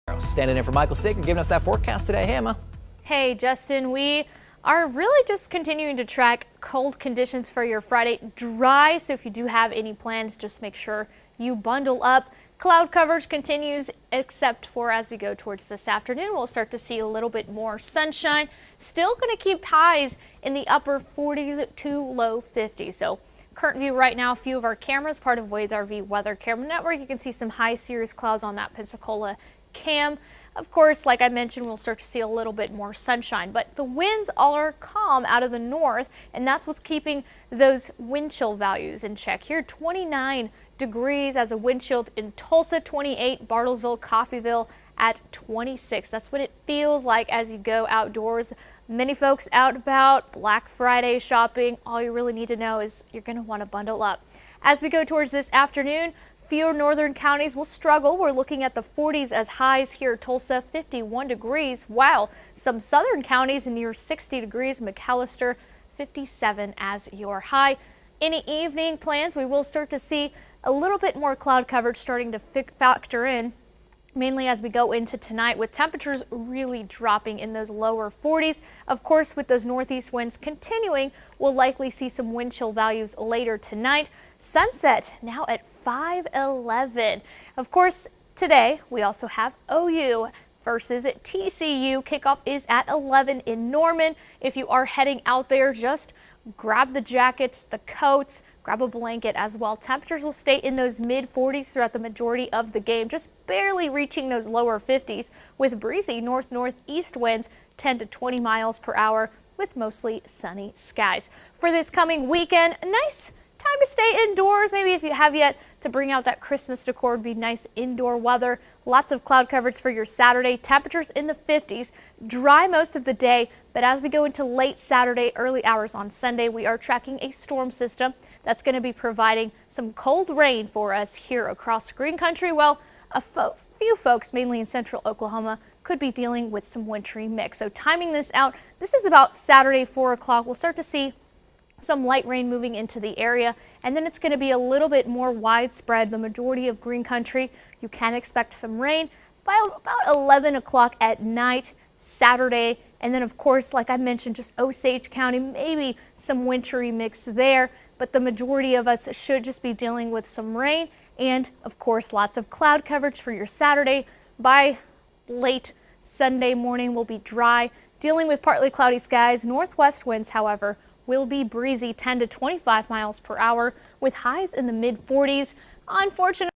Chilly Friday
Meteorologist Emma Landeros has your forecast for Nov. 24, 2023.